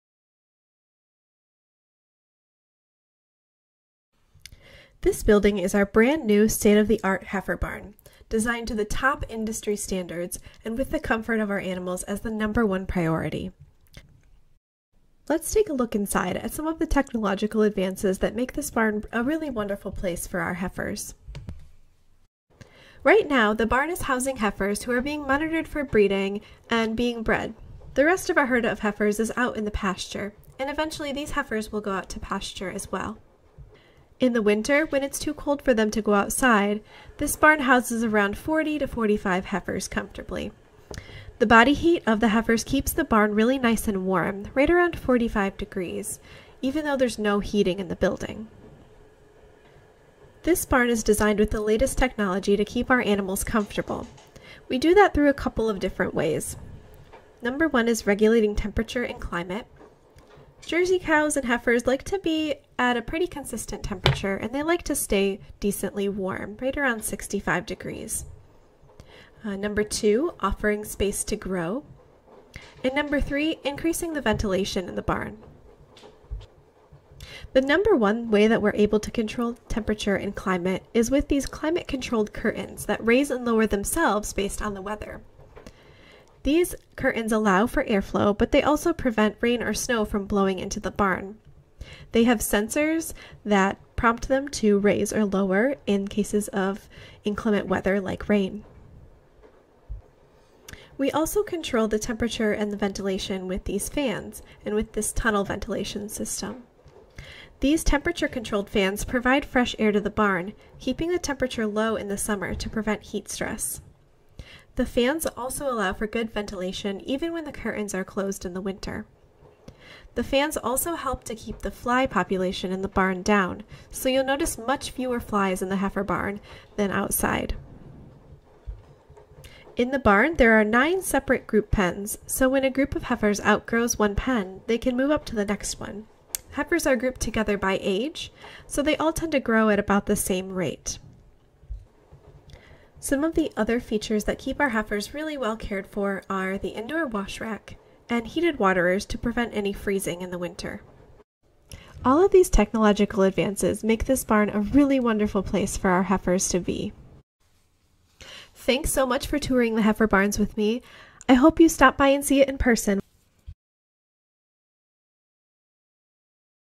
Billings Farm at Home: Tour the New Heifer Barn
Join Christine on a tour of the new state-of-the-art Heifer Barn at Billings Farm. Completed in 2019, it is now open to the public. Find out what features make it such a great home for our cows.

There's more to learn at Billings Farm at Home.